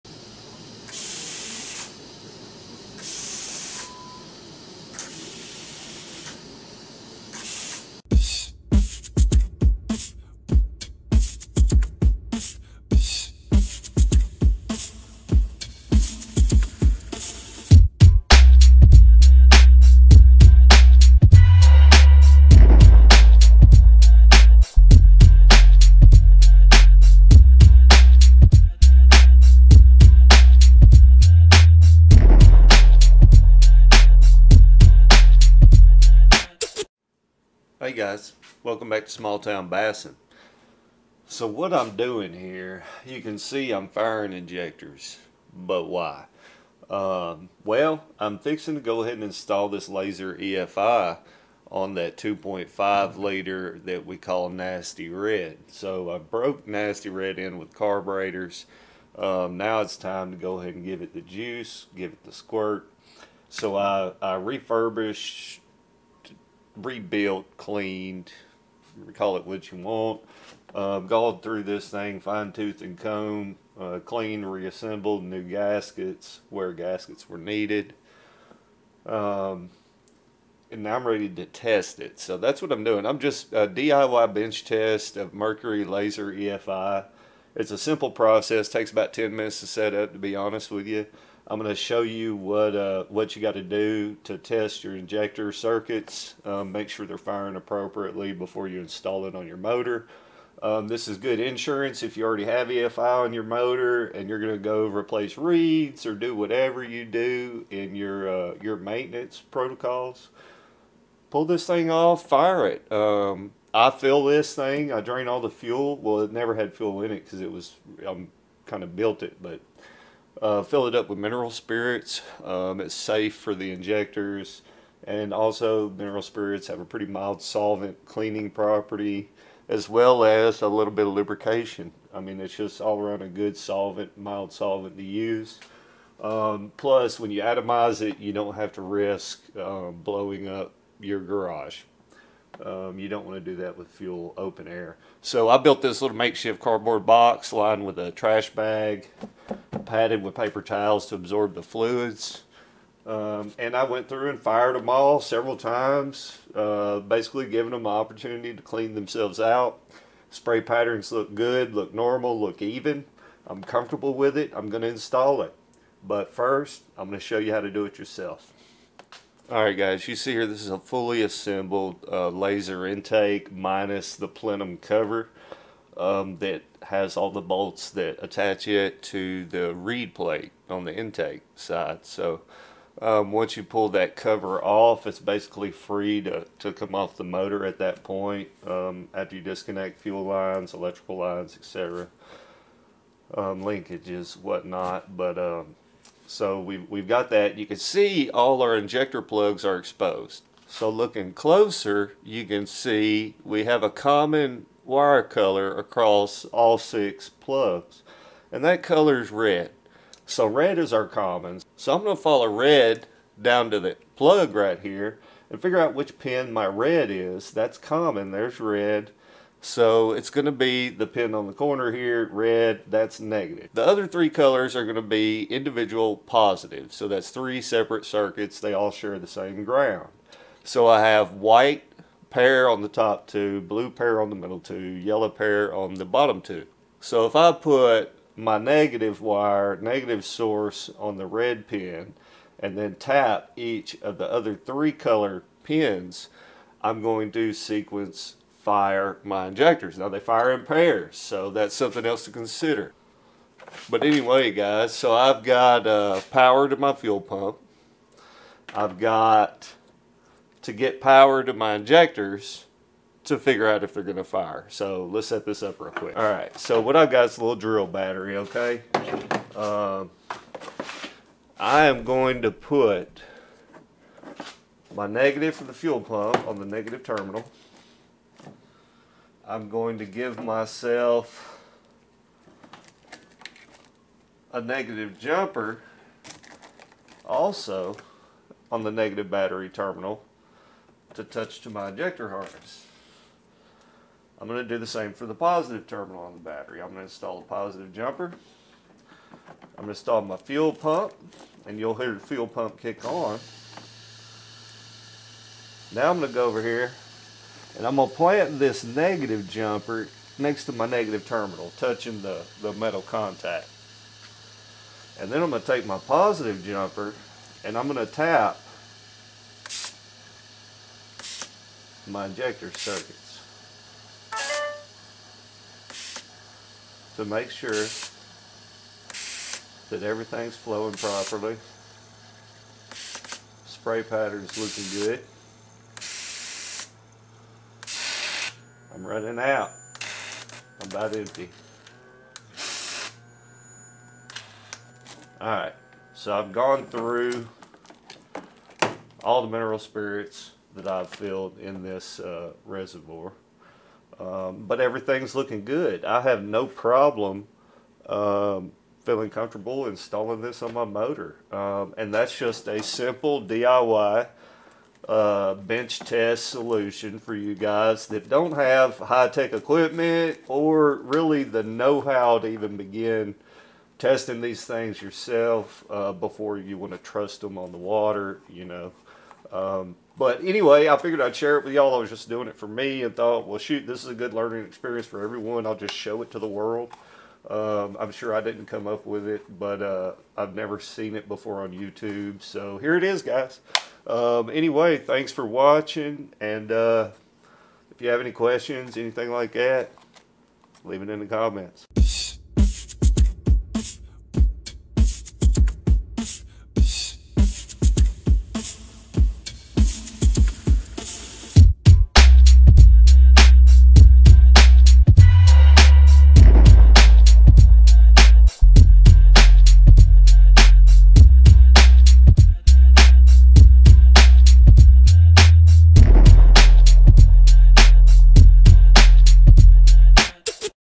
HOW TO - MERCURY LASER EFI BENCH TEST
Test it before you trust it. In this video i setup a quick bench test for my freshly assembled MERCURY LASER ELECTRONIC FUEL INJECTION. Using mineral spirits as a cleaning/testing fluid i describe in detail how to test your injector circuits using a simple drill battery as a power supply.